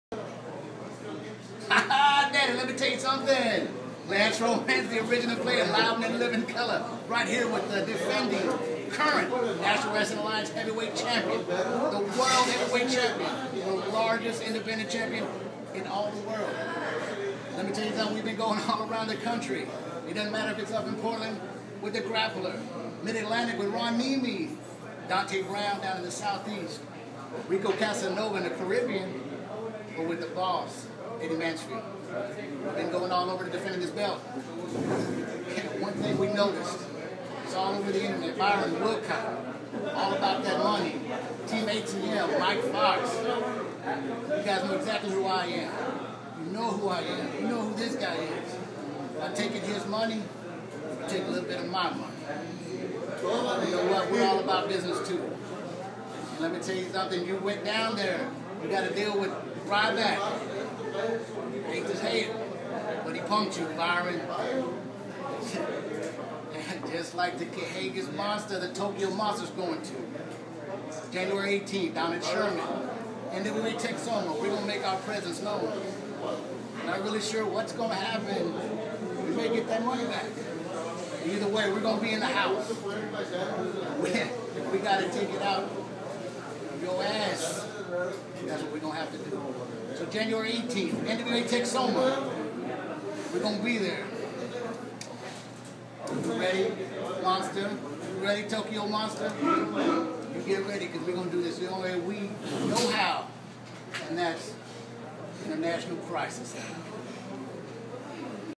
Tokyo Monster Kahagas w/ Lance Romance Respond To Wilcott &
NWA world Heavyweight Champion Tokyo Monster Kahagas w/ International Crisis Manager Lance Romance Warns Team All That Money "ATM" Byron Wilcott and Mike Foxx and Will Be At NWA Texoma: Jan 18, 2013 Sherman, Texas. Your Messing With The Wrong Envelope!